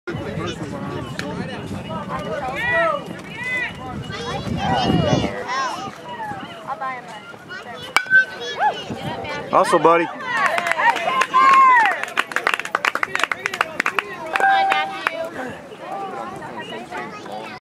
2011-05-29_Chandler_Cole_Hit_RBI
Chandler Cole of the SA Longhorns hits a grounder down the 3rd base line for an RBI single.